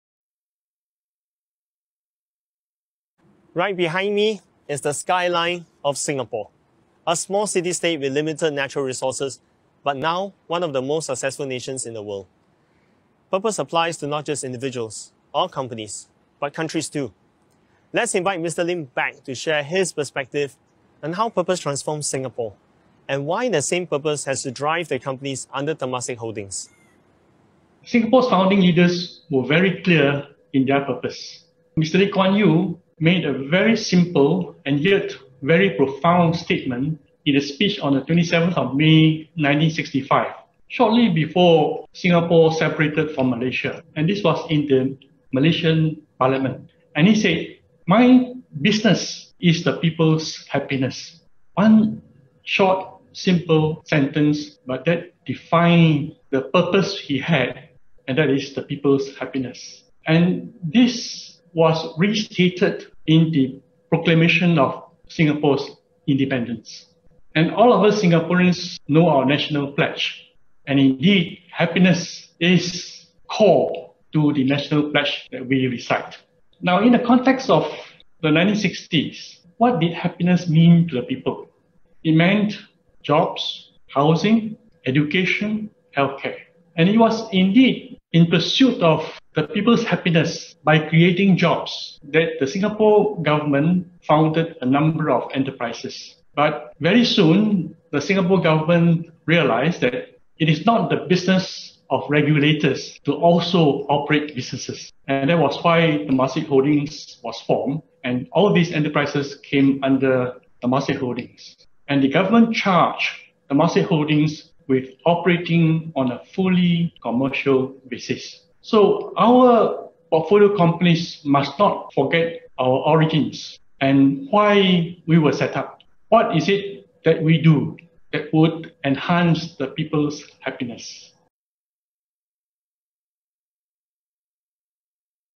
How Purpose Transformed Singapore | Lim Boon Heng | WorldView
A Singapore National Day Special! In this 2-min sharing during our WorldView Programme, hear what Mr. Lim Boon Heng had to say about how Mr. Lee Kuan Yew's strong sense of Purpose guided Singapore's growth. Happy 56th Birthday to Singapore and all Singaporeans!